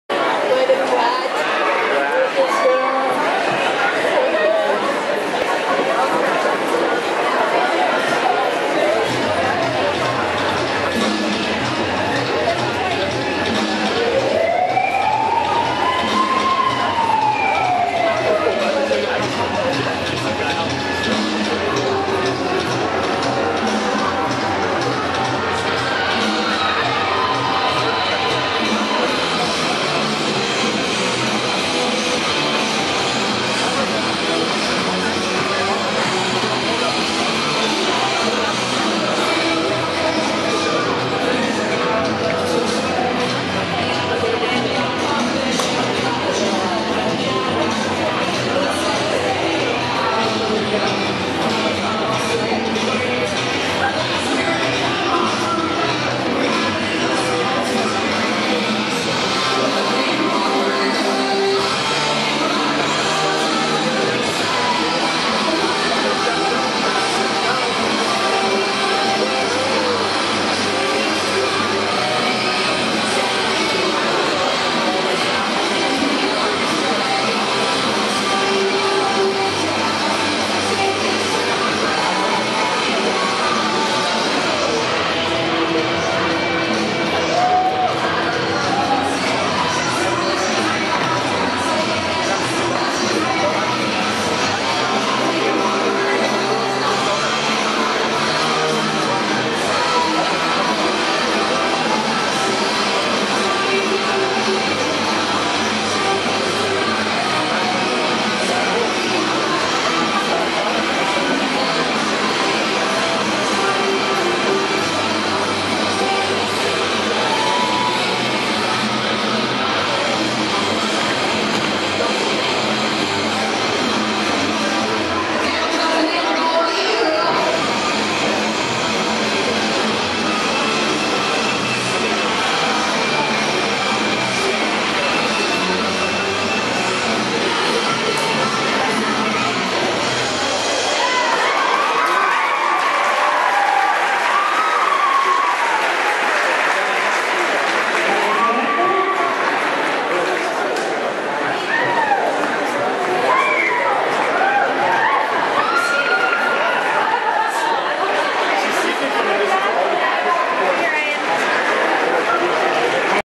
Cops and Mobsters -- Hillcrest High School @ 2012 Gem State Classic
Performed by Hillcrest High School Team (from Idaho Falls, Idaho).  Youth Swing Division 2.

*** Event:  2012 Gem State Classic (Friday, Feb. 24th, 1:15PM to about 11:30PM) -- Featuring the USA Dance National Show Dance Championships (a USA Northwest Regional Youth Ballroom/DanceSport Competition for Grades K-12).  Held at Madison High School in Rexburg, Idaho (home of BYU-Idaho).  See it again next year on March 22-23, 2013 (live in person or on webcast at EISNLive.com).

19 Competing Ballroom Studios and Schools (with multiple teams) -- from 5 states (Idaho, Nevada, Oregon, Utah, and Washington):